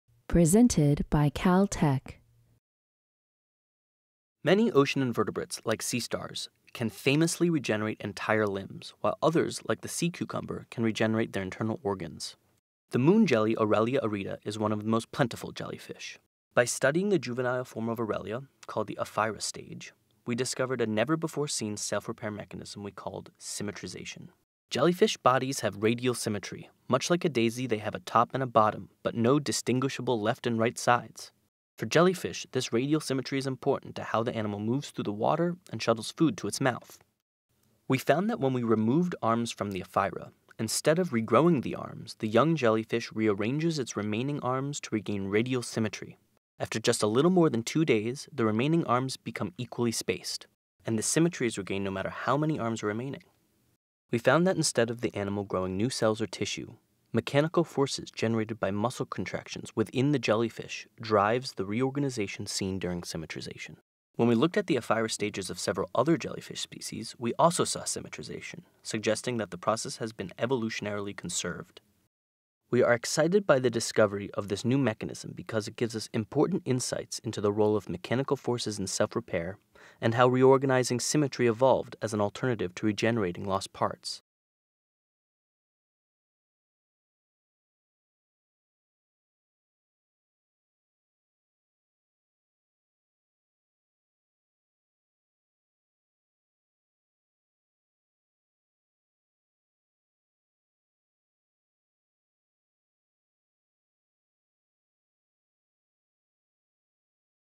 Self-Repair in Jellyfish - Goentoro Lab - June 2015
Researchers at the Goentoro Lab at the California Institute of Technology have discovered a previously unobserved self-repair mechanism in juvenile jellyfish. Paper: "Self-repairing symmetry in jellyfish through mechanically driven reorganization", published in the June 15th online edition of Proceedings of the National Academy of Sciences (PNAS). Contributors to this work include Michael Abrams, Ty Basinger, William Yuan, Chin-Lin Guo, and Lea Goentoro.